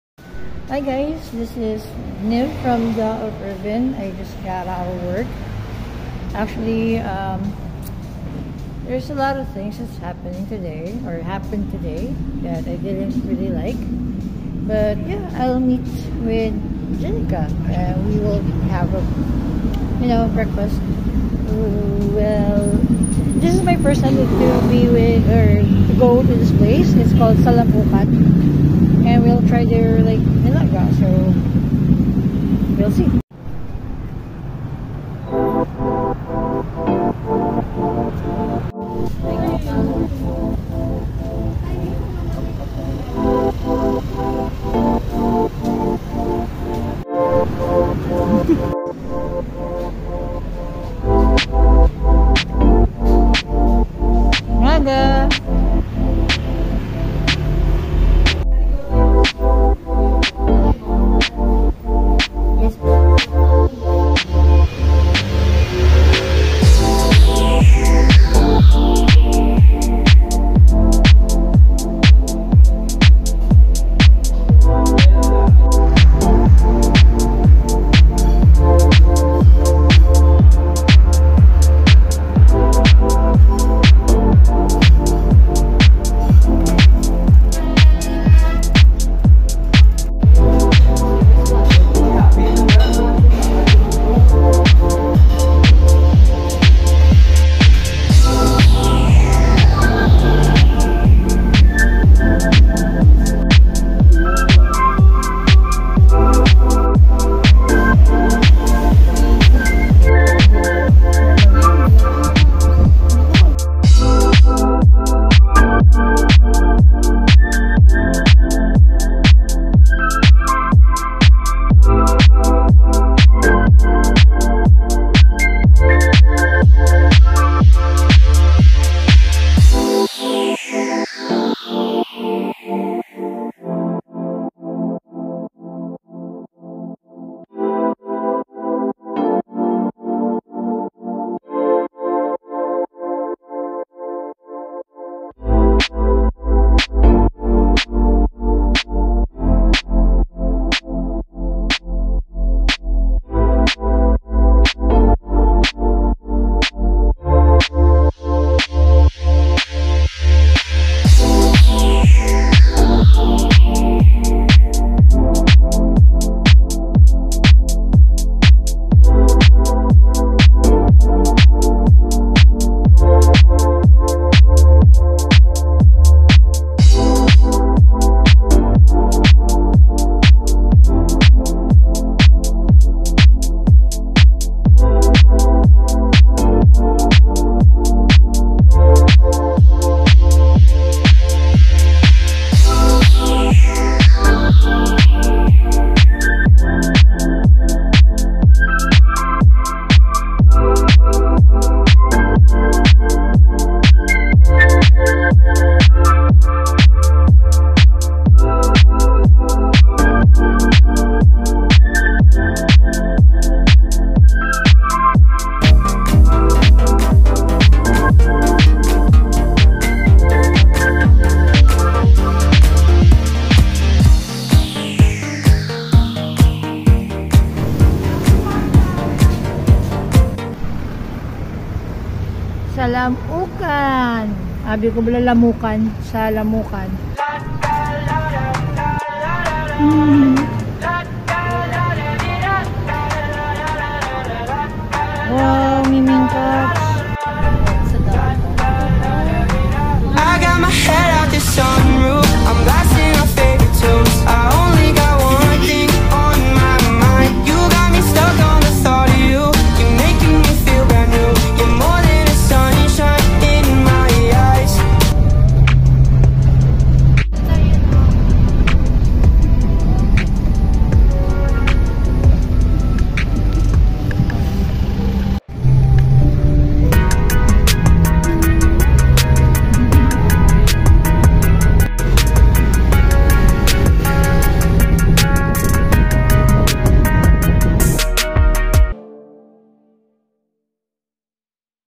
Me and My Jowa: Salam-ukan!!!
Me and Jenica went to Salam-ukan to eat nilaga and just enjoy. Not toys but foods.